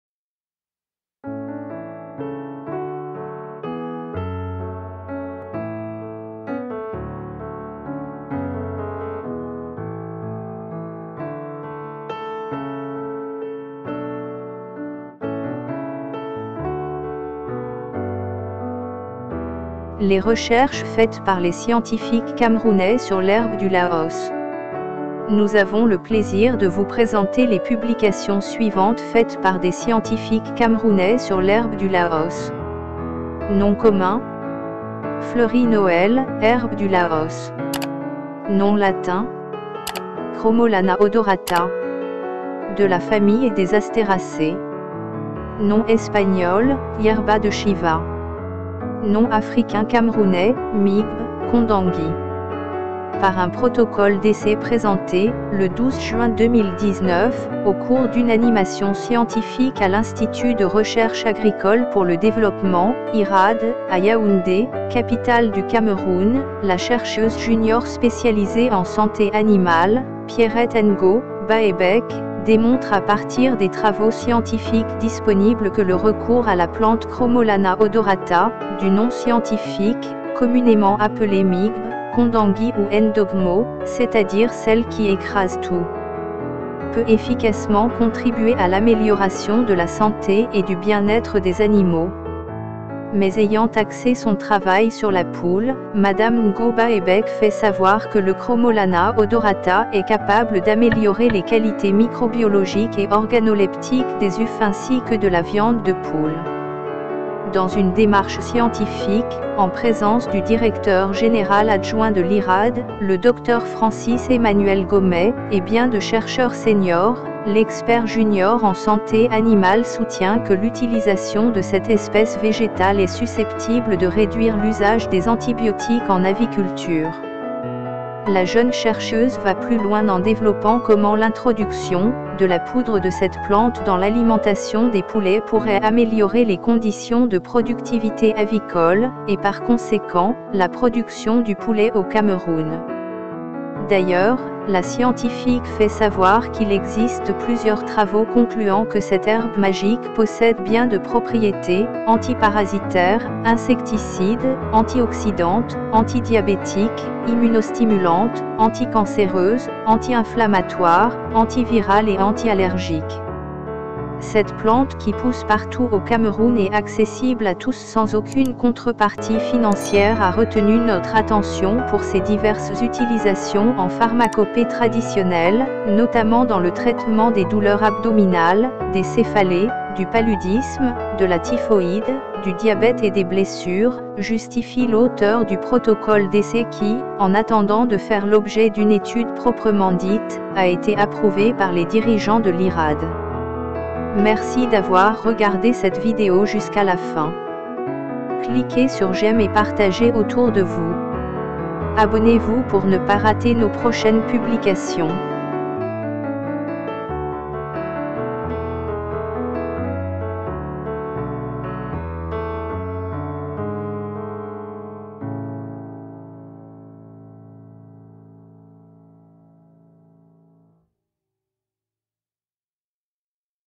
L'usage de L'herbe du Laos dans l'élevage
Le fleurit-noël est un arbuste à tige lignifié dont la hauteur peut varier suivant le sol et le climat où il se trouve, cela va de moins d’un mètre à près de huit mètres, il fait partie de la famille des astéracées.
Ses branches sont étalées avec un feuillage persistant d’un vert clair et dont les feuilles si on les froisse parfument l’air de térébenthine voir de kérosène, elles sont simples et opposées.
Son inflorescence est réunie en capitule terminal fait de fleurs blanches à bleu clair.
Le fleurit-noël est originaire d’Amérique tropicale, il est installé en Floride jusqu’en Argentine, il est présent également aux Antilles ainsi qu’en Inde et au Cambodge, ainsi qu’au Vietnam et à Taiwan, puis en Australie et en Afrique du Sud.